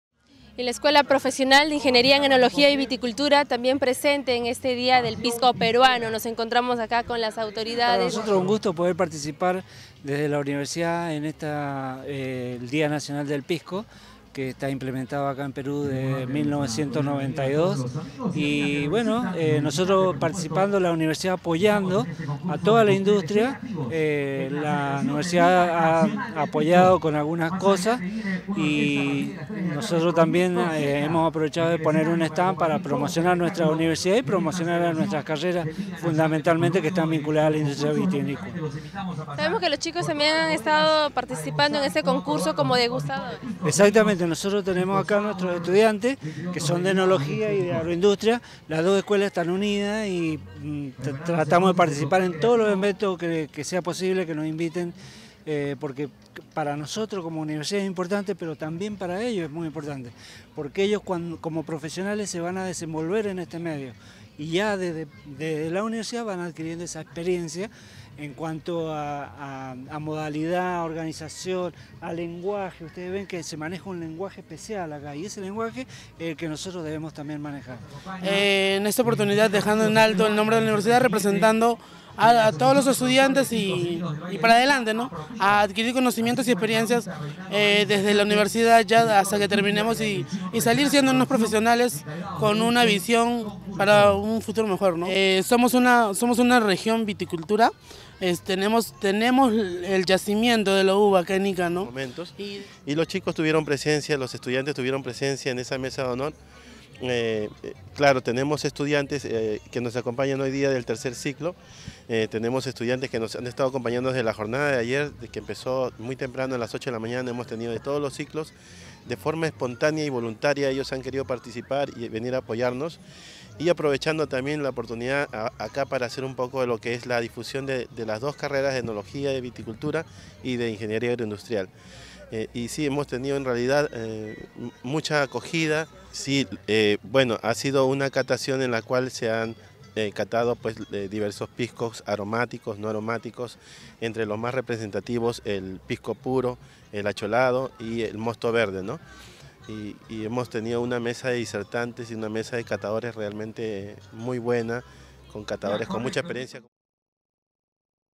UPSJB Y LA ESCUELA ENOLOGIA PRESENTES EN EL DÍA DEL PISCO.
LA ESCUELA PROFESIONAL DE INGENIERÍA EN ENOLOGÍA Y VITICULTURA DE LA UNIVERSIDAD PRIVADA SAN JUAN BAUTISTA PARTICIPO EN LA CELEBRACIÓN POR EL  DIA DEL PISCO PERUANO, LOS  ESTUDIANTES FORMARON PARTE DEL CONCURSO DE CATA SOMETIENDO SUS SENTIDOS PARA EL  ANÁLISIS Y CALIFICACIÓN DE NUESTRA BEBIDA BANDERA.